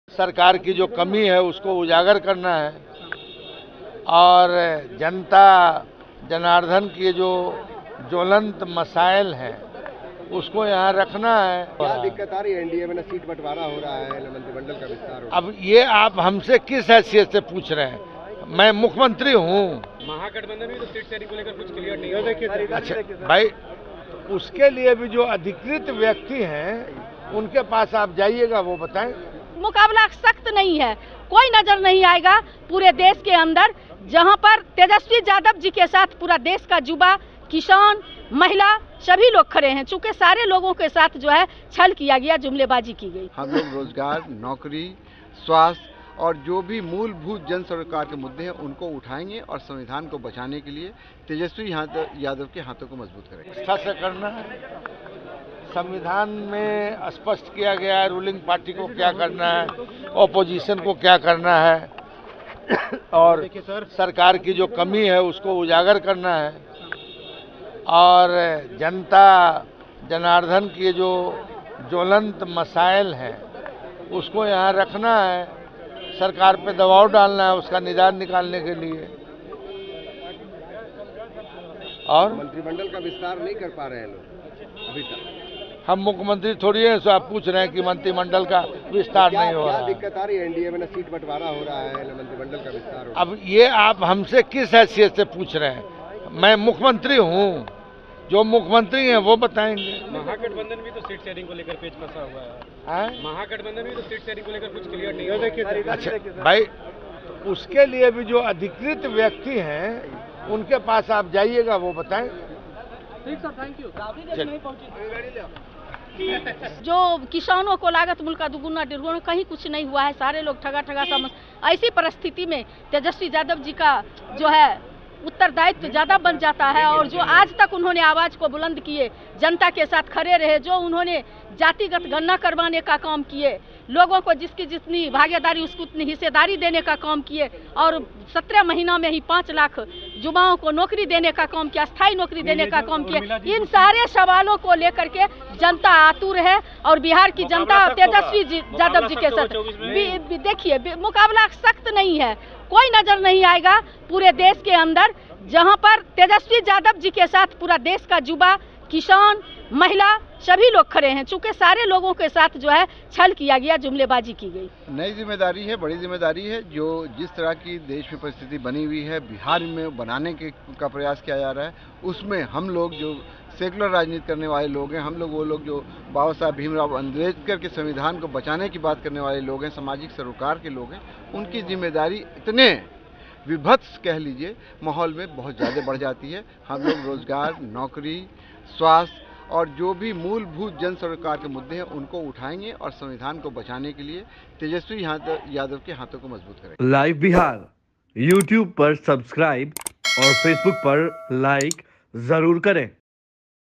RJD के नवनिर्वाचित MLC ने भाजपा के खिलाफ खूब बोले, कहा-गरीबों के लिए हमलोग आवाज उठाते...
Live Bihar Digital Network